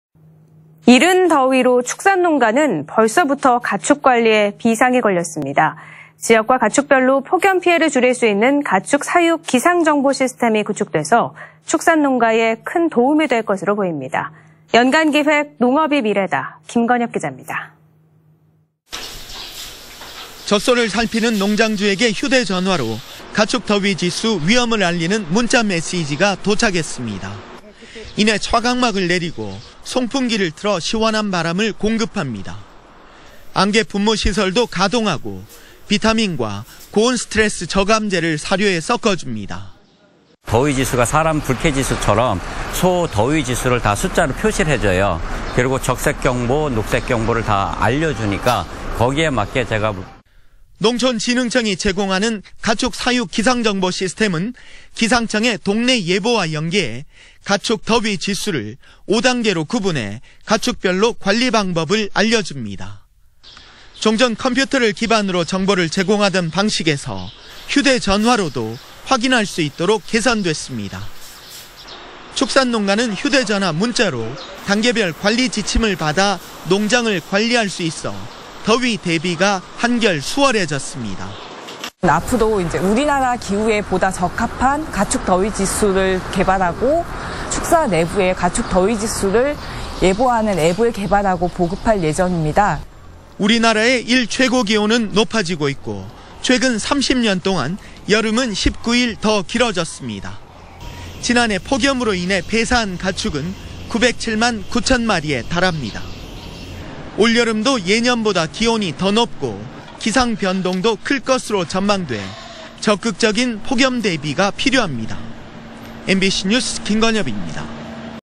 폭염 피해 막는 '가축 사육 기상정보시스템' 구축 / 안동MBC
2019/06/06 15:35:45    작성자 : 김건엽

◀ＡＮＣ▶
이른 더위로 축산농가는 벌써부터
가축 관리에 비상이 걸렸는데요,

지역과 가축별로 폭염 피해를 줄일 수 있는
'가축 사육 기상정보시스템'이 구축돼
축산농가에 큰 도움이 될 것으로 보입니다.

연간기획, 농업이 미래다 김건엽기자
◀ＥＮＤ▶

젖소를 살피는 농장주에게 휴대전화로
가축 더위 지수 '위험'을 알리는 문자 메시지가 날아 왔습니다.

이내 차광막을 내리고 송풍기를 틀어
시원한 바람을 공급합니다. 안개분무시설도
가동하고 비타민과 고온스트레스 저감제를
사료에 섞어줍니다.

◀ＩＮＴ▶ 조용기/축산농민
"더위 지수가 사람 불쾌지수처럼 숫자로
다 표시해줘요. 적색, 녹색 경보를 알려주니까
거기에 맞게 제가..."

[C/G]농촌진흥청이 제공하는
가축 사육 기상정보시스템은 기상청 동네예보와
연계해 가축 더위지수를 5단계로 구분해
가축별 관리방법을 알려줍니다.

종전 컴퓨터를 기반으로 정보를 제공하던
방식에서 휴대전화로도 확인할 수 있도록
개선됐습니다.

축산농가는 휴대전화 문자로
단계별 관리지침을 받아 농장을 관리할 수
있어 더위 대비가 한결 수월해졌습니다.

◀ＩＮＴ▶ 이유경 농촌진흥청 연구사
"앞으로 우리나라 기후에 보다 적합한
가축 더위 지수를 개발하고 축사 내부의
가축 더위 지수를 예보하는 앱을
(추가로) 보급할 예정입니다."

우리나라의 일 최고기온은 높아지고 있고
최근 30년동안 여름은 19일 더 길어졌습니다.
지난해 폭염으로 인해 폐사한 가축은
907만 9천 마리에 달합니다.

올 여름도 예년보다 기온이 더 높고
기상 변동도 클 것으로 전망돼
적극적인 폭염 대비가 필요합니다.
MBC뉴스 김건엽입니다.